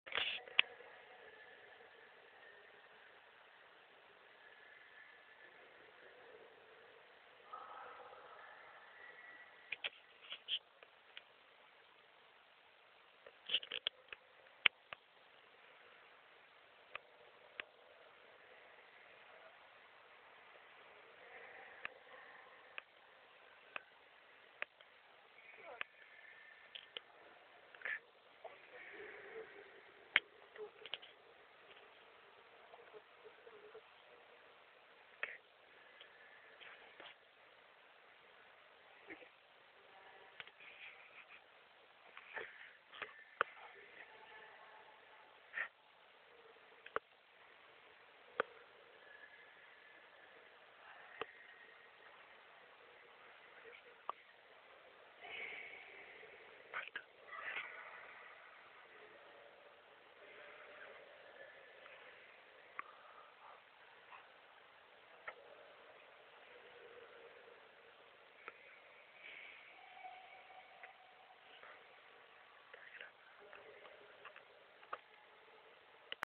iglesia de tulancingo
una grabacion dentro de la iglecia de tulancingoo,